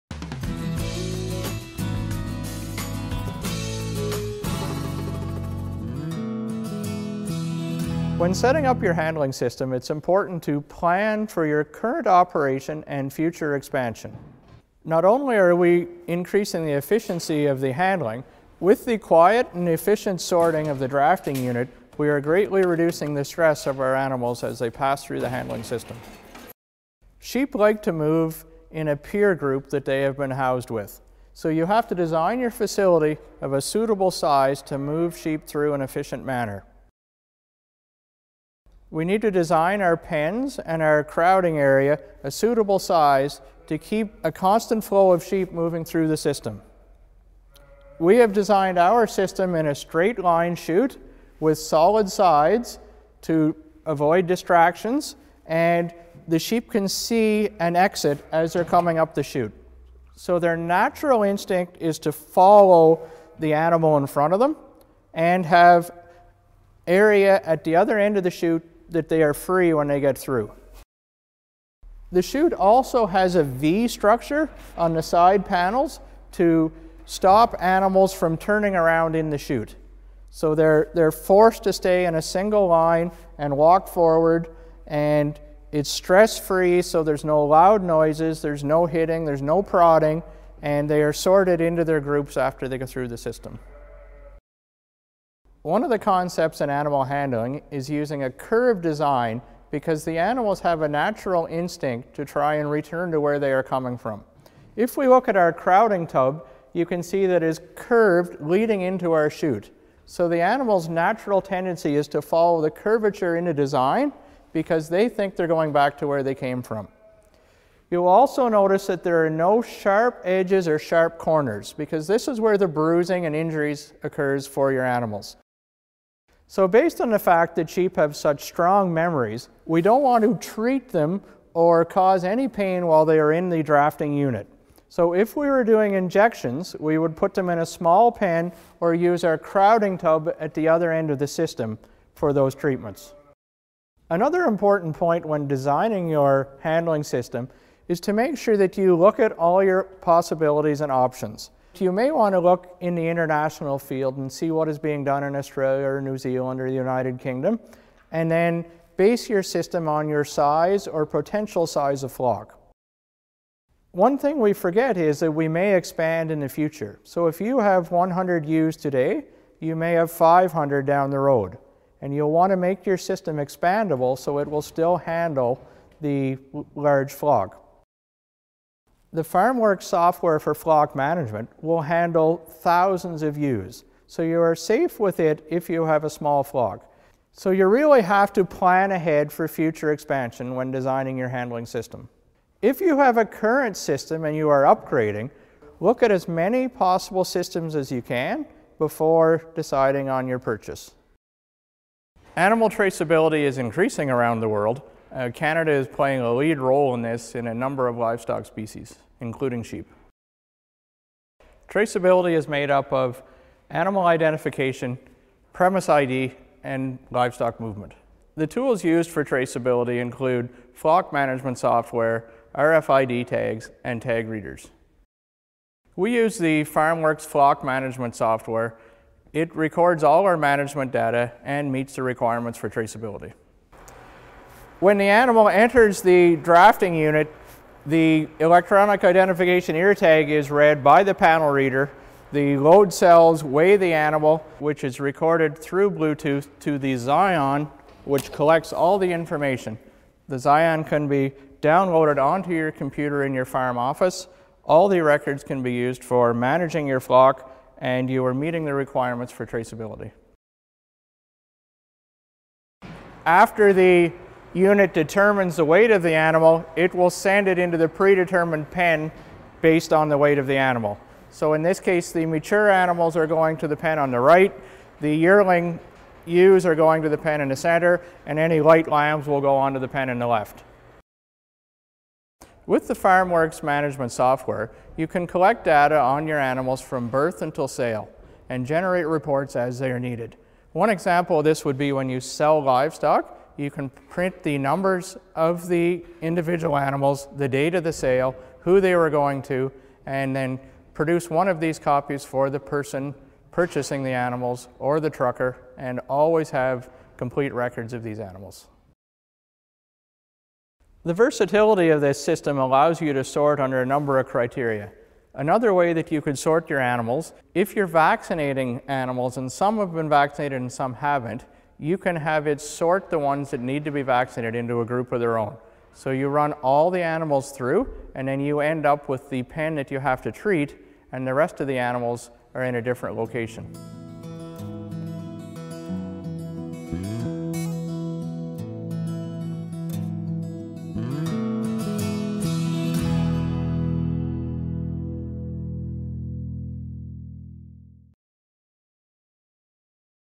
Handling Systems - Lakeland College
Since 2007 Alberta pilot projects have focused on looking for flock management benefits in moving to national traceability and in implementing new electronic technology, RFID (radio frequency identification) tags, tag readers and flock management equipment.
The Lakeland College handling and data management system is one example.  By planning a system that incorporated new technology and existing facilities the College improved flock management.  The system makes day-to-day work faster and easier; sorting and grouping of sheep, routine weighing, pregnancy detection, and managing health treatments.  It also makes animal handling safer and less stressful for both students and sheep.